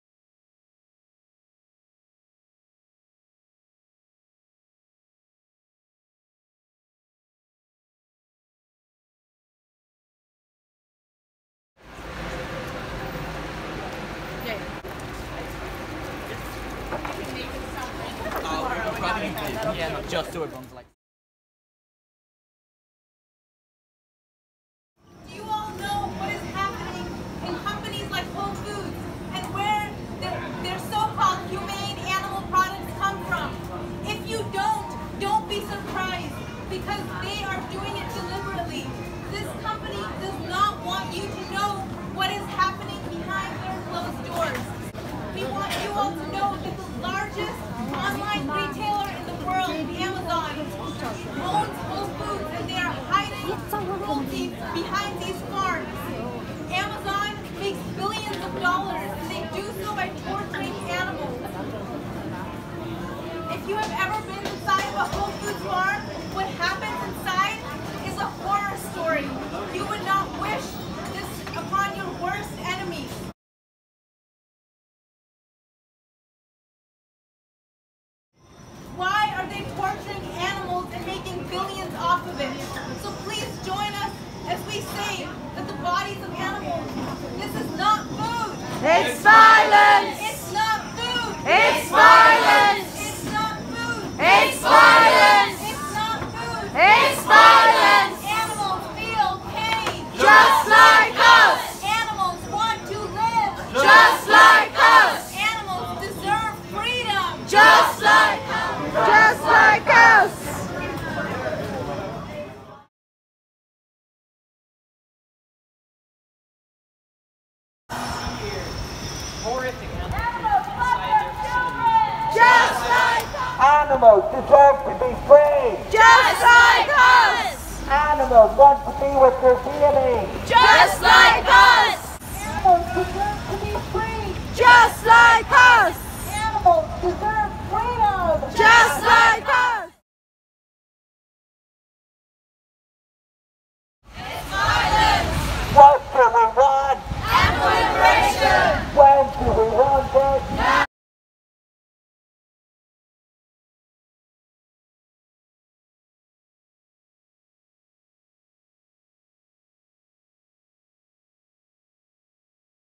Direct Action Everywhere Disruption At Whole Food Market
DxE-UK join forces with Direct Action Everywhere - DxE and DxE Direct Action Everywhere - Northern Ireland in taking over London Cities Whole Foods Market featuring Animal Activists Almira Tanner and Priya sawhney from the USA to deliver a message to the stores management and the deceived public about the crimes Big Ag has committed and just how people are going to expose these crimes to the world!
Truth Matters-Do not buy the humane lie!